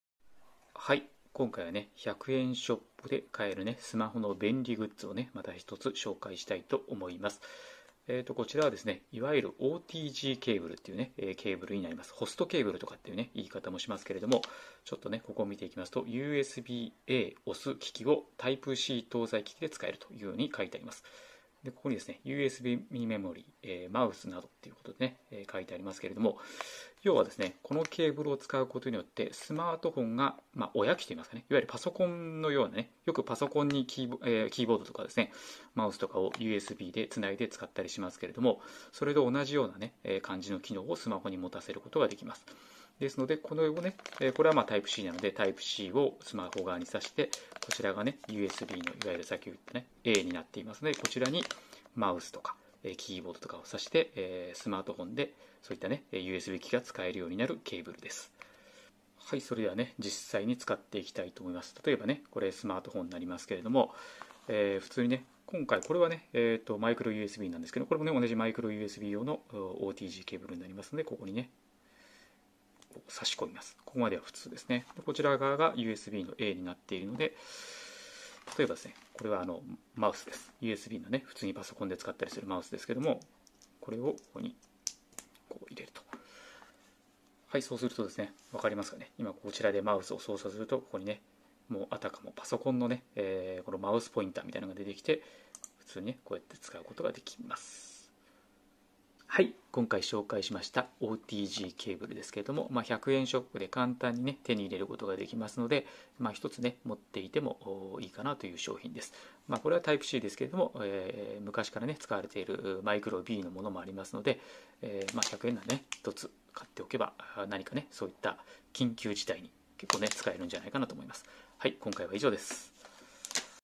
スマホでマウスなどのUSB機器が使える便利グッズ OTGケーブル 100円ショップで買えます！
スマホにUSB機器を接続可能にするケーブルOTGケーブルの紹介です。
マウスやキーボード、USBメモリなどUSB機器を利用可能になります。
100円ショップでも手に入れることができるので、一つ持っていて損はありません。
※一部OTGに非対応の端末もあります。